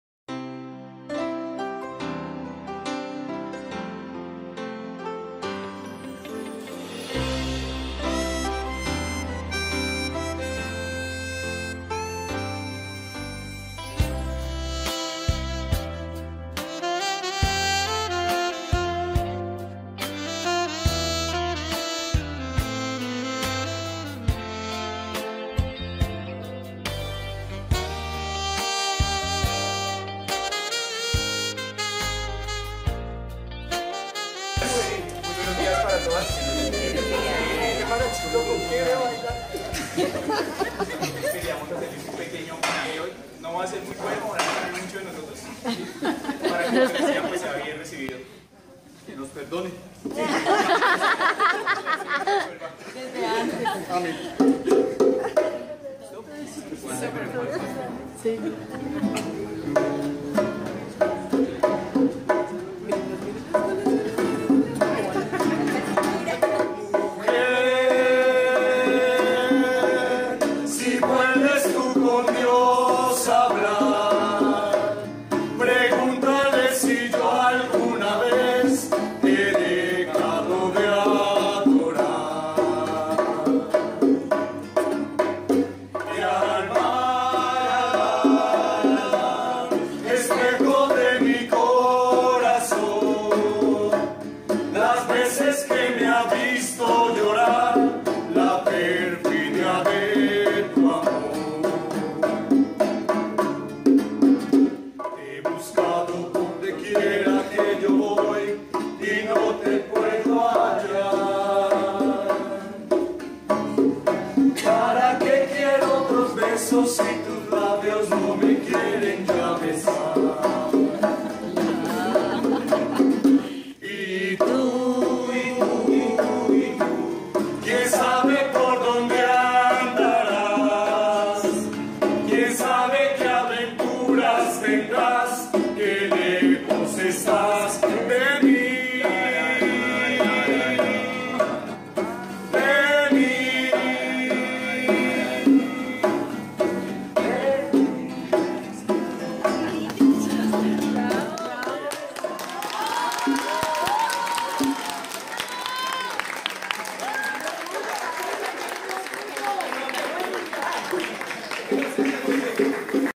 Celebración día de la mujer Colegio Hernando Durán Dussán IED
Hermosa celebración de nuestros compañeros hombres a las mujeres de la institución en su día!!!